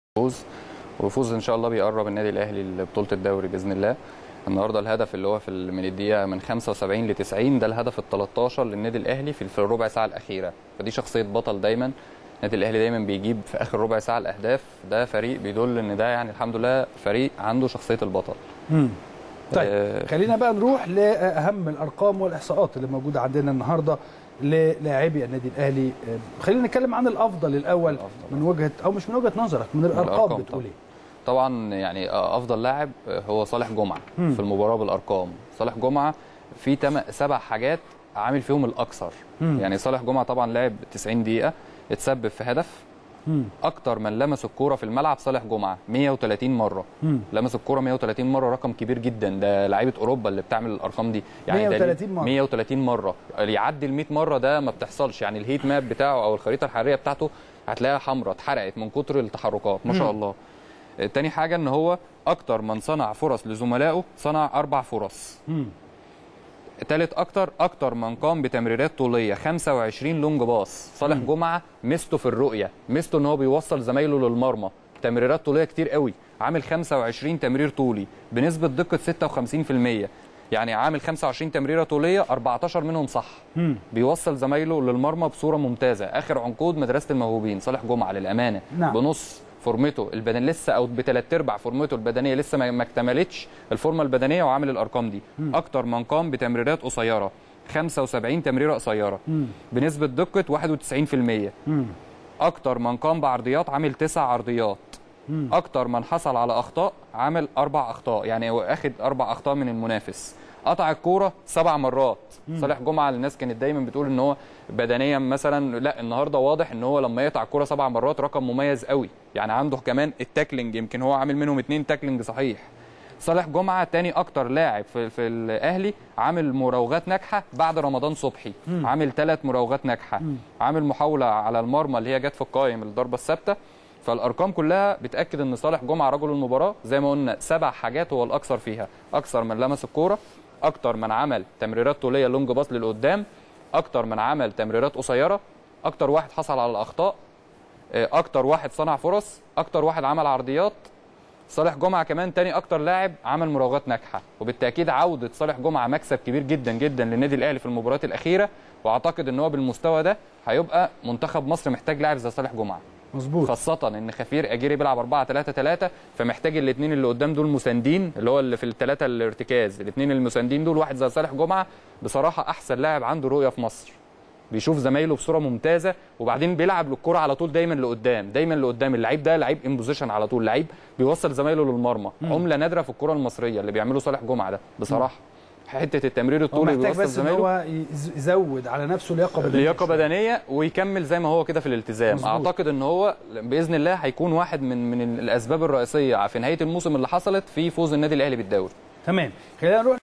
احمد ثابت : صالح جمعة كانت تحركاته مثل لاعب اوروبى لدرجة ان الخريطة الحرارية "هتتحرق"
احمد ثابت : صالح جمعة كانت تحركاته مثل لاعب اوروبى لدرجة ان الخريطة الحرارية "هتتحرق" من كتر التحركات

التردد: 12341 أفقي.
القمر: نايل سات.
معامل الخطأ: 5/6.
معدل الترميز: 27500